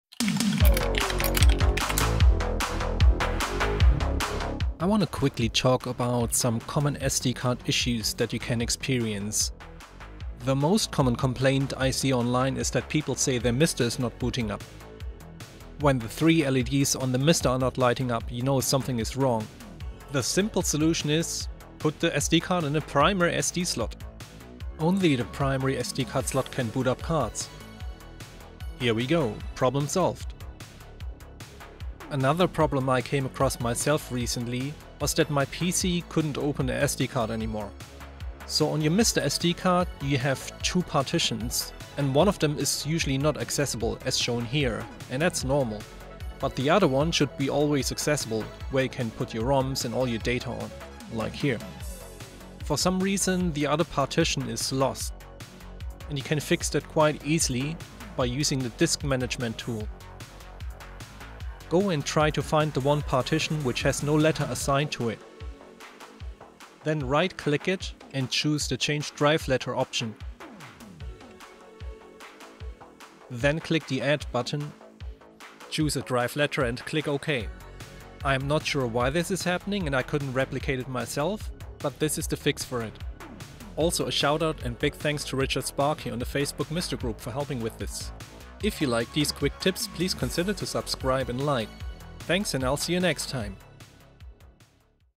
MiSTer FPGA | How to Fix SD Card Issues | Quick Tips Guide
How to fix two SD card issues you may come across with MiSTer FPGA.

Chapters:
00:10 - Mister not booting
00:31 - SD Card not accessible on PC

Music licensed through Artlist. Copying and/or reproducing this content in any form without written permission is prohibited.